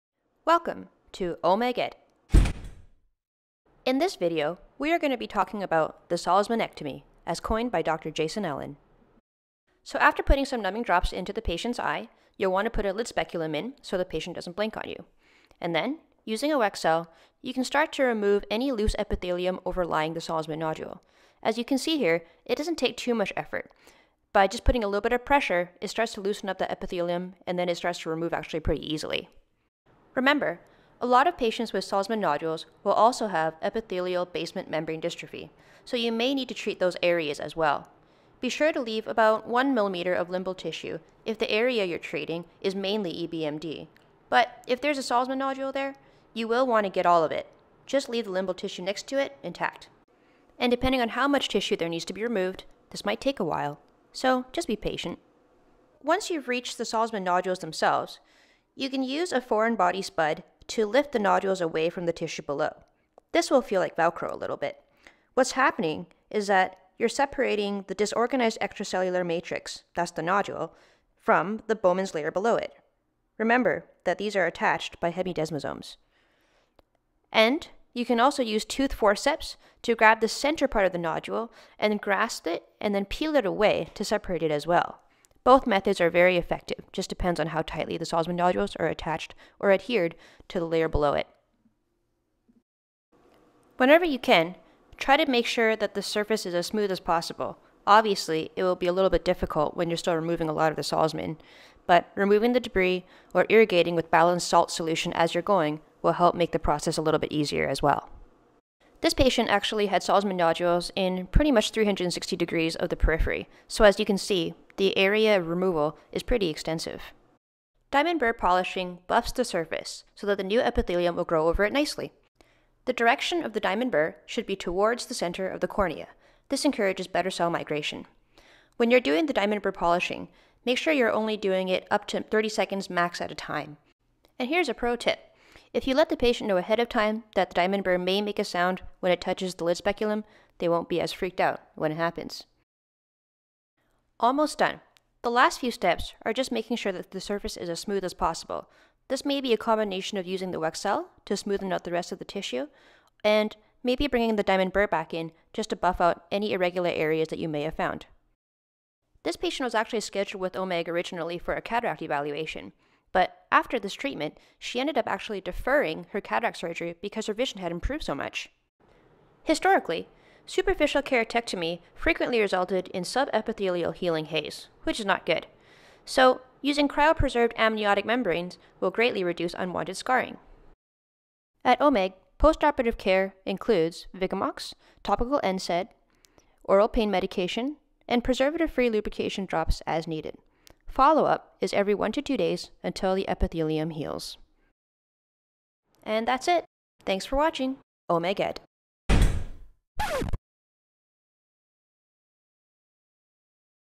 The "Salzmannectomy" - as coined by Dr. Jason Ellen
In Oklahoma, optometrists have the privilege to treat Salzmann nodular degeneration.  A corneal curettage, diamond burr polish, and insertion of a cryopreserved amniotic membrane is used to treat this common condition that increases dry eye symptoms, irregular astigmatism, and can significantly affect premium cataract surgery outcomes.

Narrated by Sophia Leung, OD, FAAO